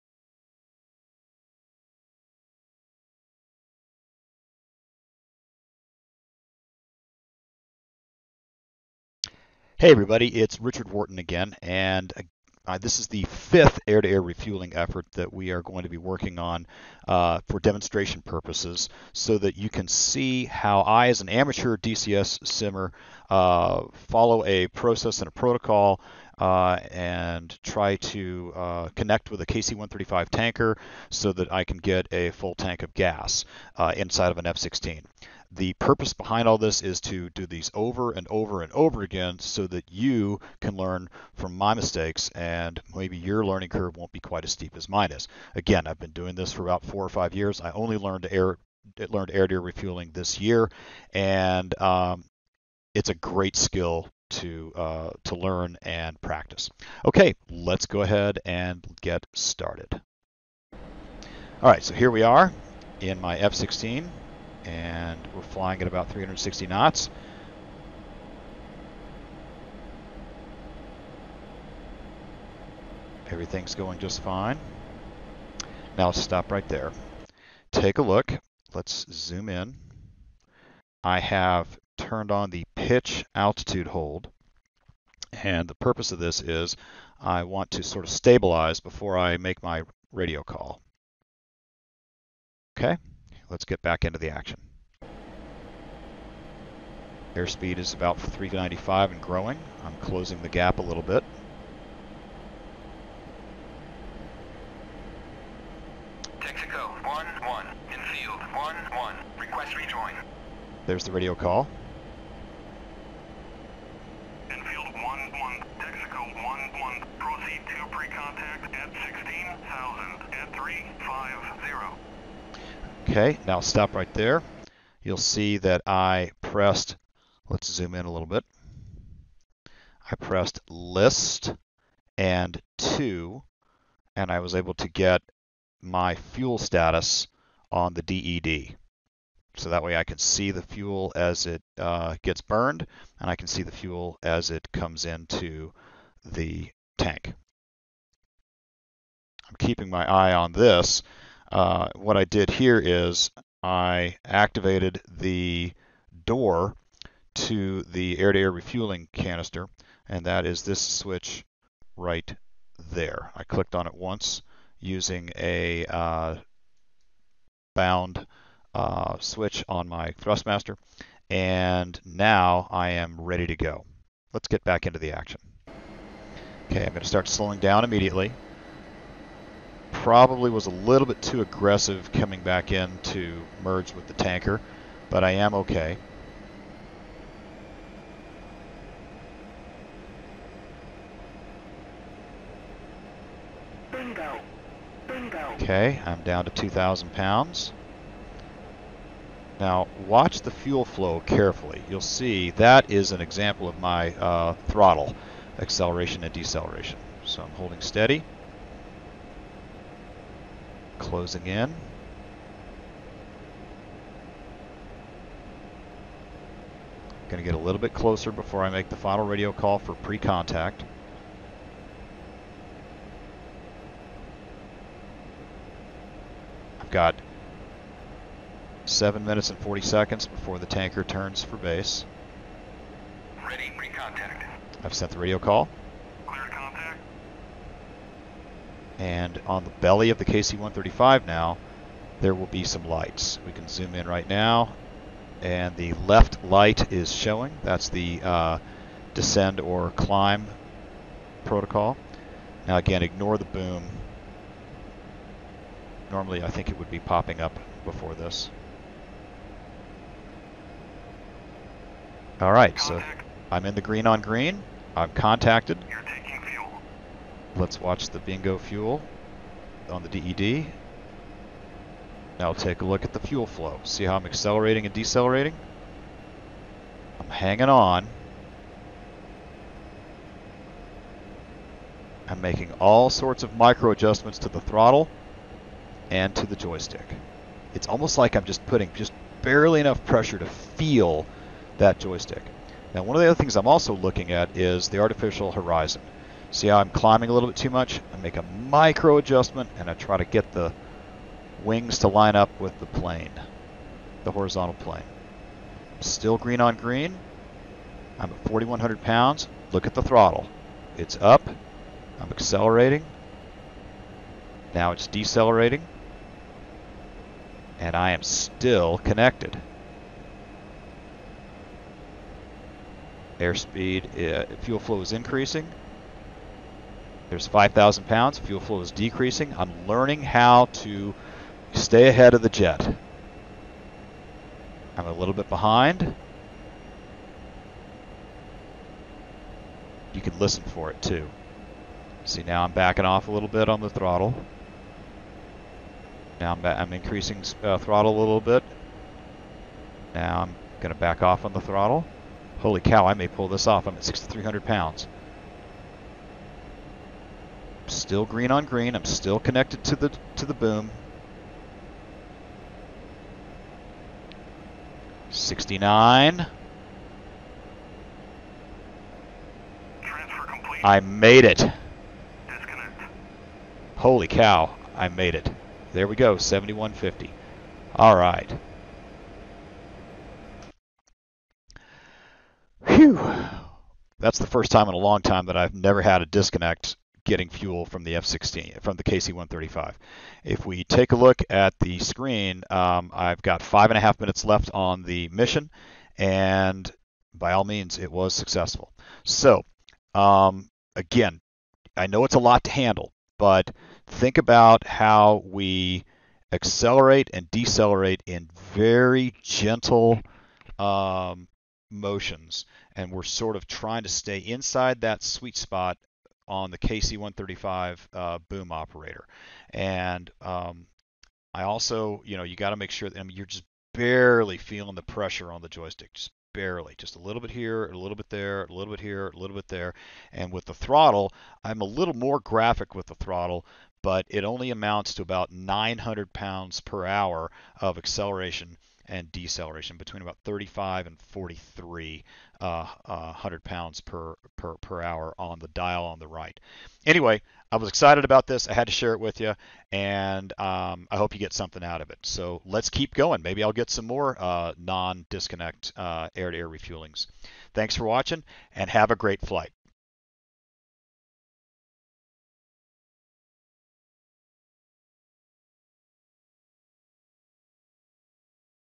F-16 Air-to-Air Refueling Video #4
Well, I connected, and then held the connection for the FULL TANK!